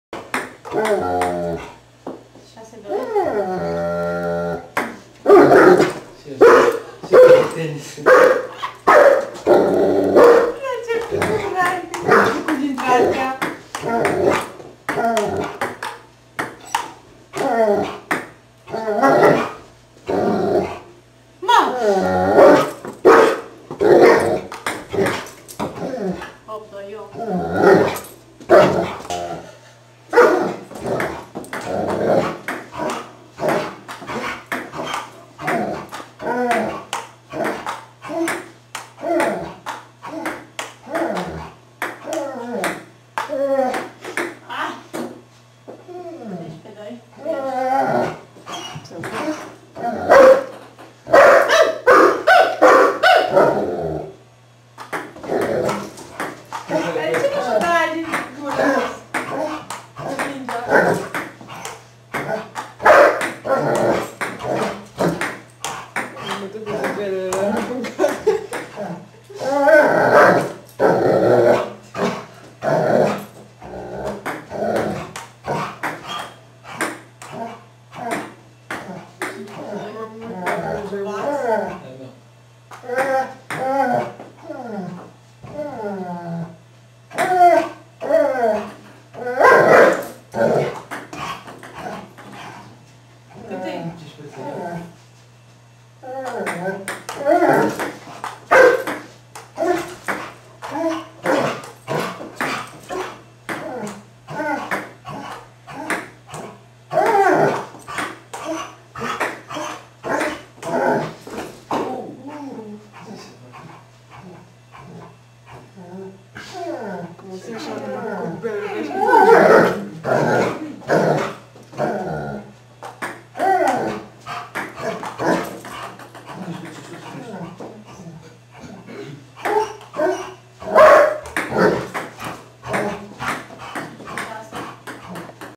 Safir -ping pong madness - July 2008
Our latest vacation in the mountains. Ellijay, GA. Jul 2008. First time Safir experienced the fun sounds and movements of ping pong :)